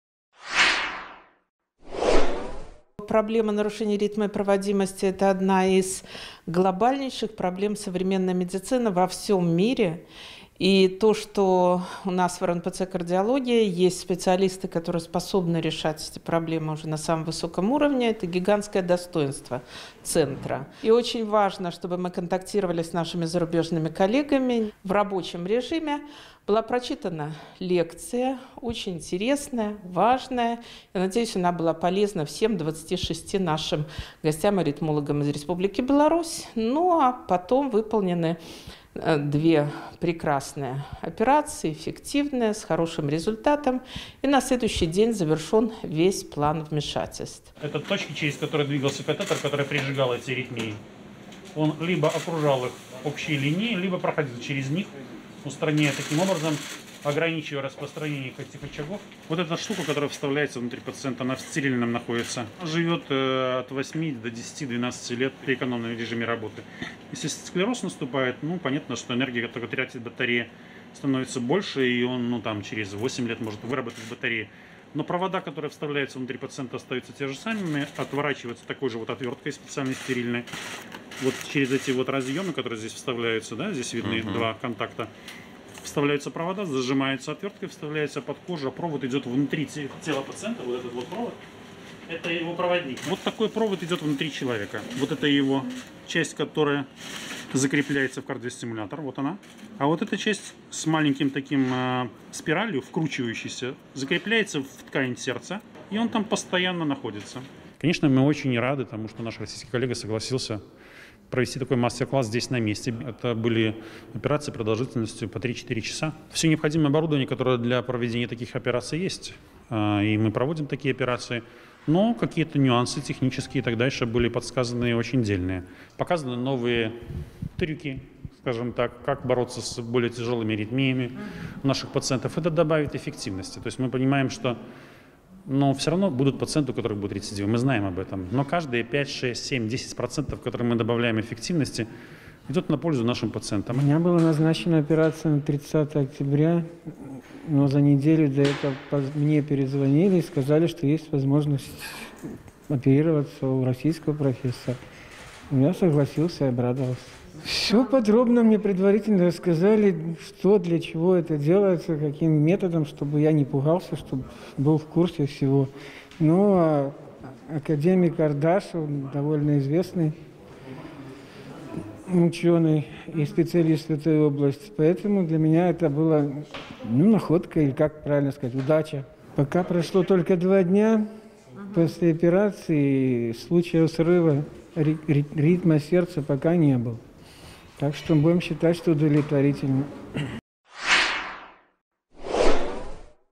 Четырех сложнейших пациентов прооперировали в РНПЦ "Кардиология"
Четырех сложнейших пациентов с нарушением ритма и проводимости сердца прооперировали в РНПЦ "Кардиология" в Минске, сообщила журналистам директор центра, председатель Белорусского общества кардиологов и терапевтов Наталья Митьковская. Операции прошли под руководством российского врача-аритмолога, президента Евразийской ассоциации аритмологов профессора Андрея Ардашева.